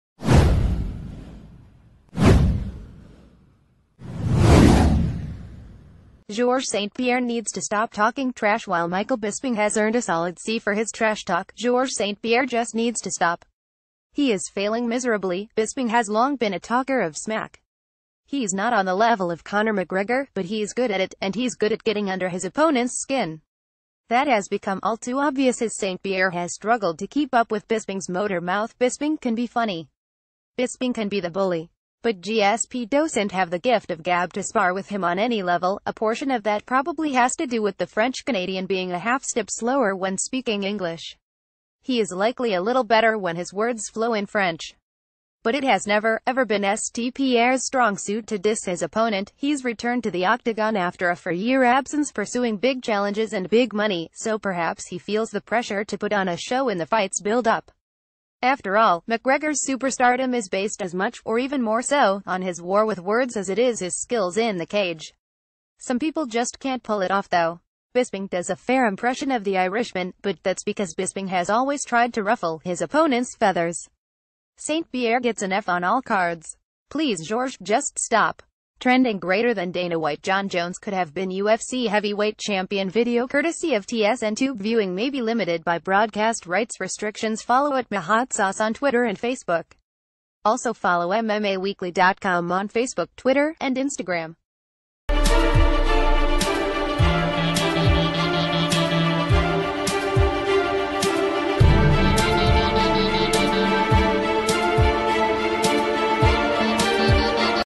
Breaking News | Georges st-pierre needs to stop talking trash | mmaweekly.com
Georges st-pierre needs to stop talking trash | mmaweekly.com

Georges StPierre Needs to Stop Talking Trash While Michael Bisping has earned a solid C for his tras...